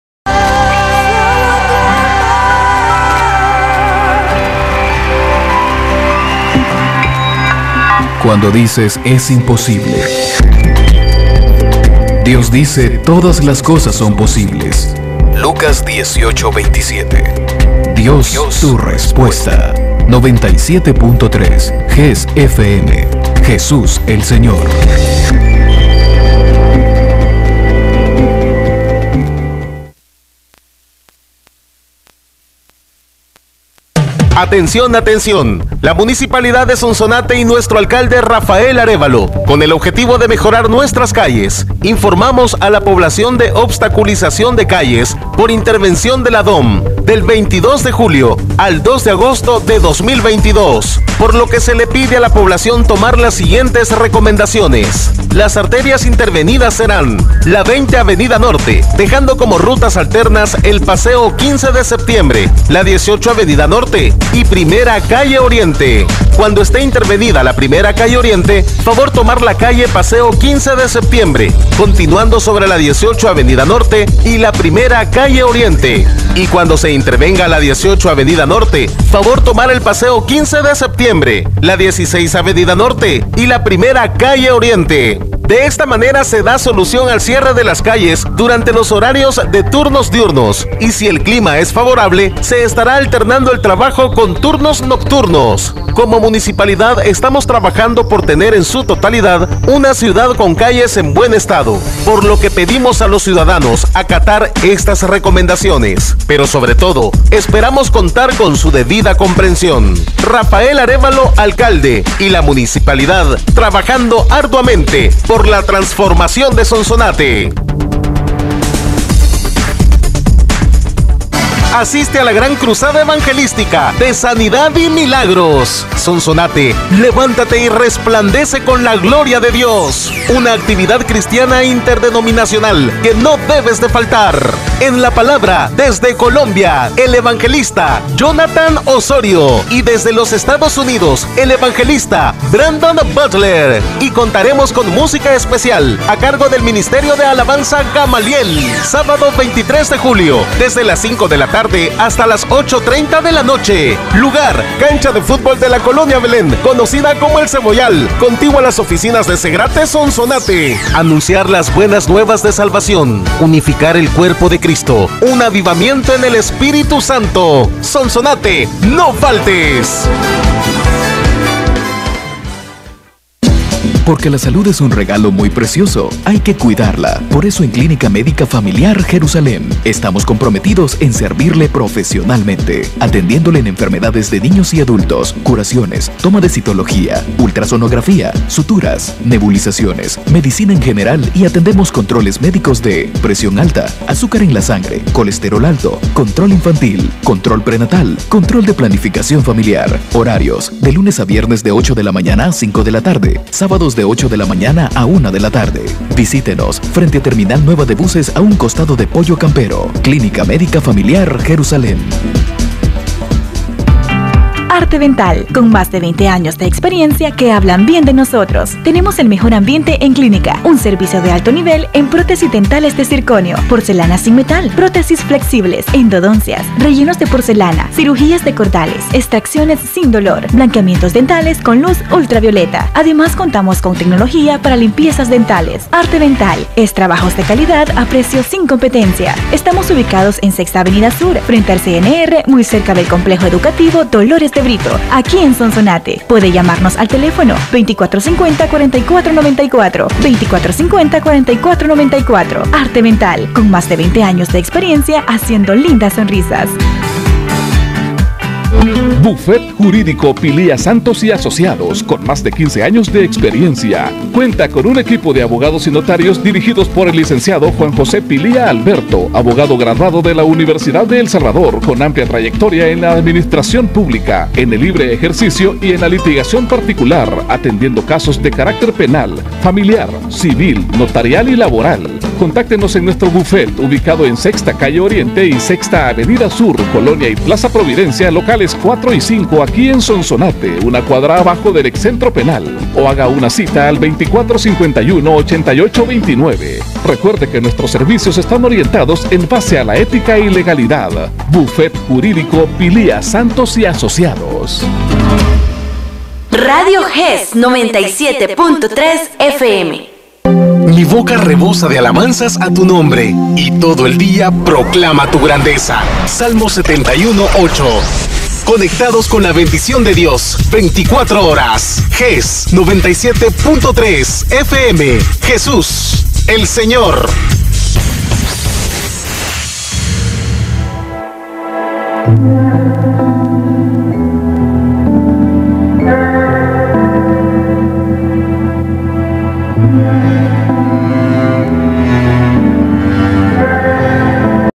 Tanda Comercial Radio Jes FM (97.3FM)(Julio 2022)
es algo casi imposible obtener comerciales en esa emisora por qué solo en fines de semana lo emiten y no durante la semana